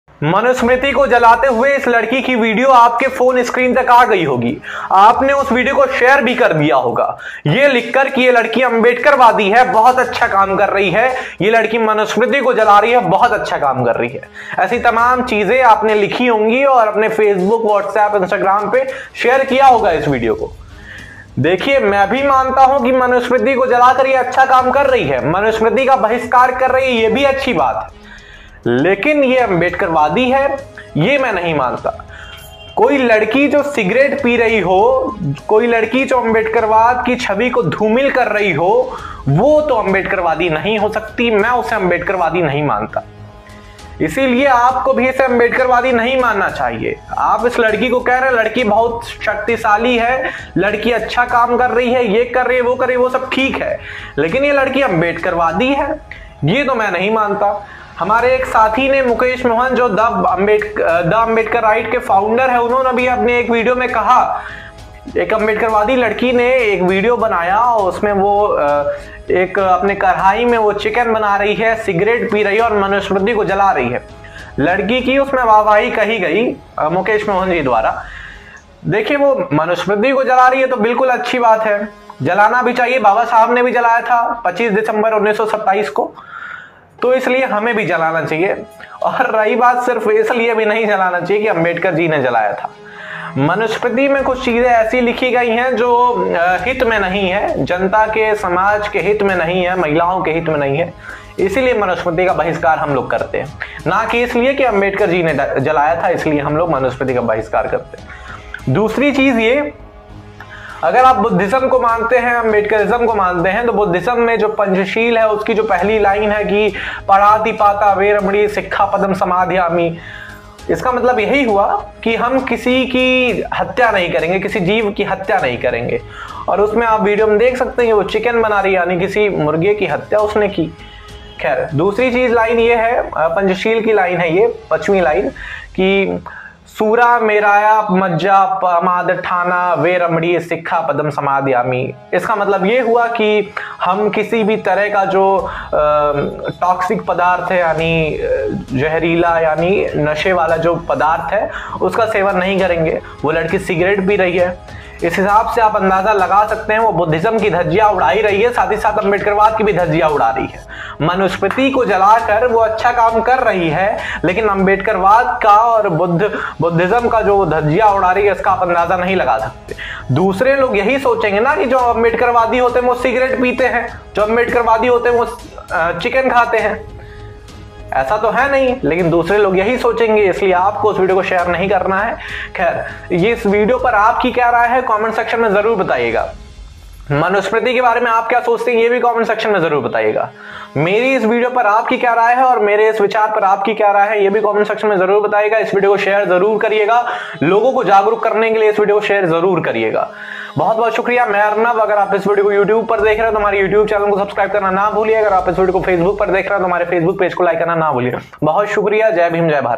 मनुस्मृति को जलाकर खुद को अम्बेडकरवादी बताना कितना सही है ? | Manuwad | Priya Viral Video Bihar
बिहार की लड़की प्रिया का वीडियो कैफ़े दिनों से वायरल है जिसमे प्रिया मनुस्मृति को जलाते दिखाई दे रही है और लो के रहे है कि वेयर अम्बेडकरवादी है अब आप बताईये